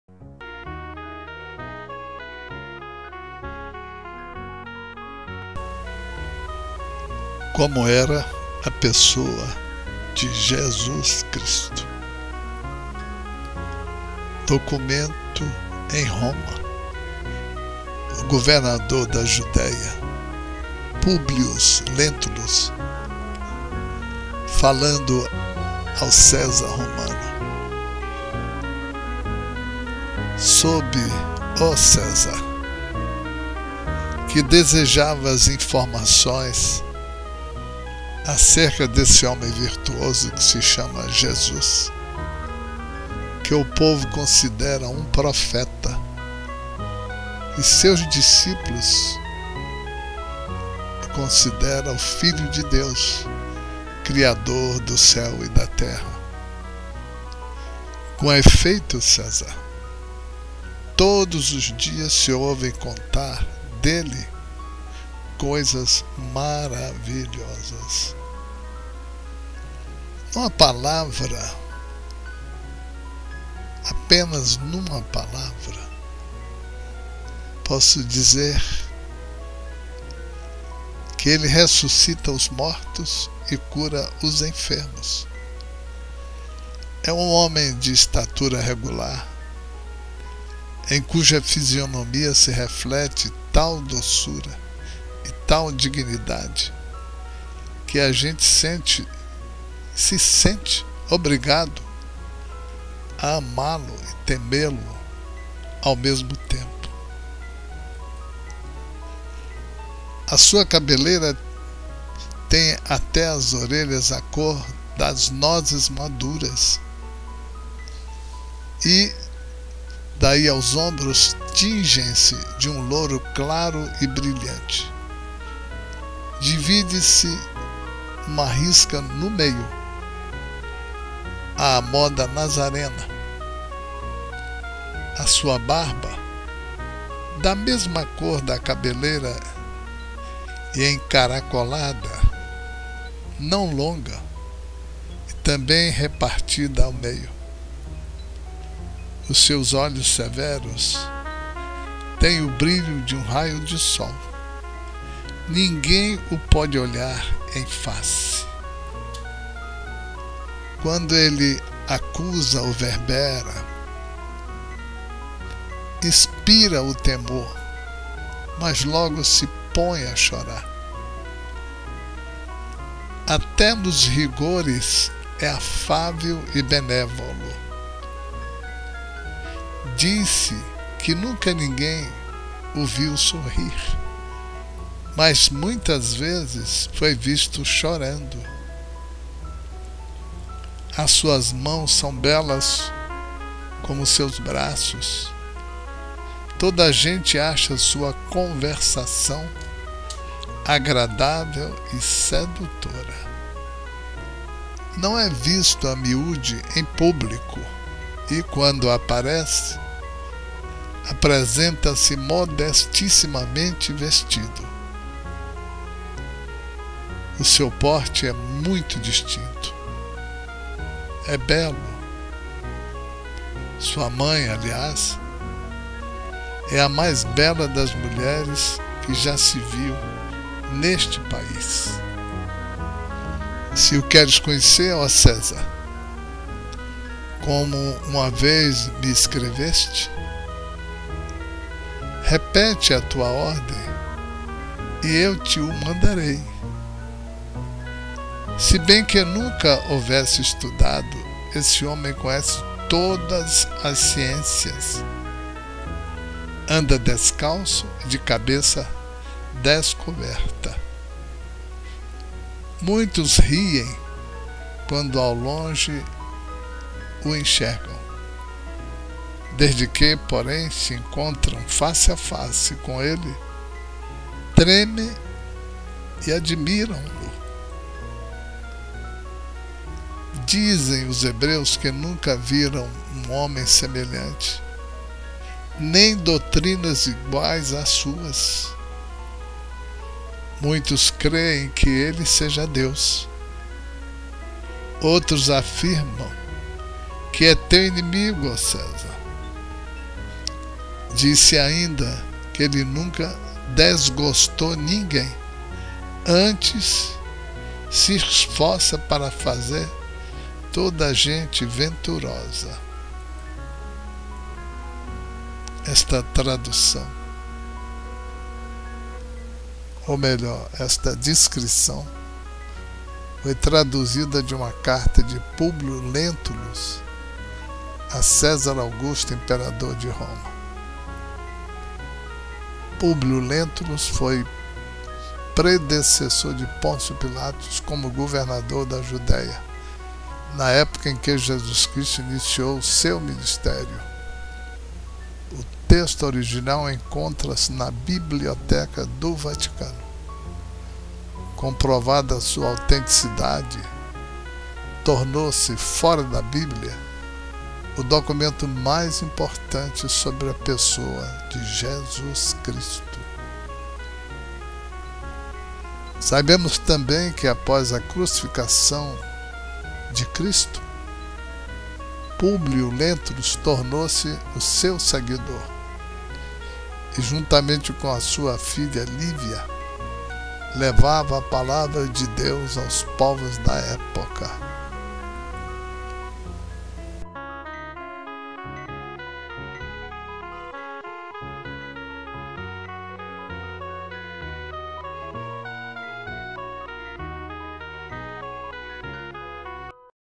COMO ERA A PESSOA DE JESUS - Documento Romano -por Publio Lentulus
Pelas inúmeras biografias deste Ser Insuperável, são claras e verdadeiras, somos hoje agraciados em cultivar em nossos espíritos a certeza de que Ele está presente em nossas vidas onde predomina o Amor. Publio Lentulus descreve isto de maneira irretocável em documento comprovadamente da época.
 Muita Paz, A.Carlos Araujo